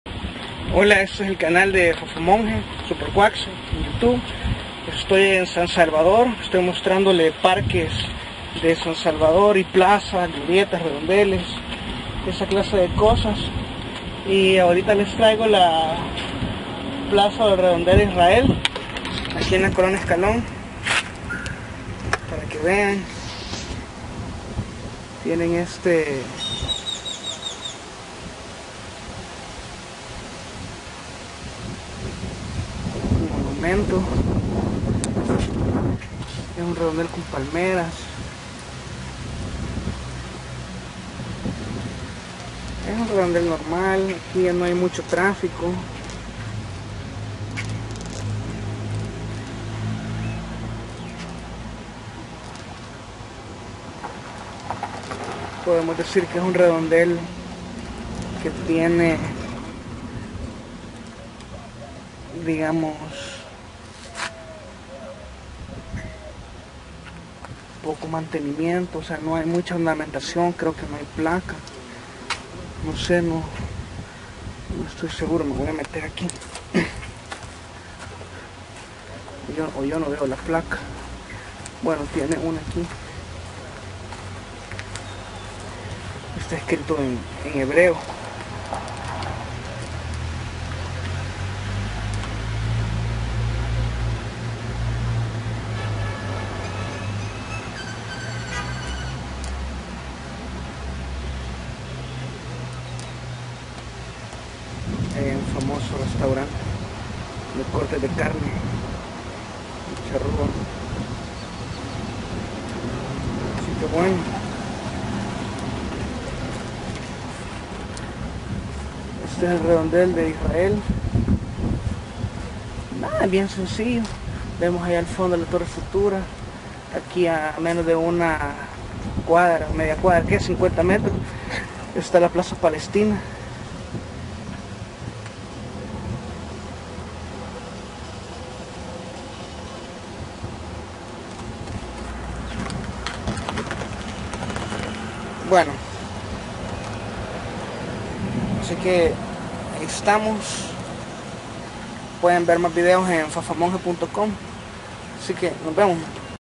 Plaza Israel (San Salvador, El Salvador)
Plaza Israel en San Salvador en la 7 Calle Poniente frente al restaurante El Charrúa.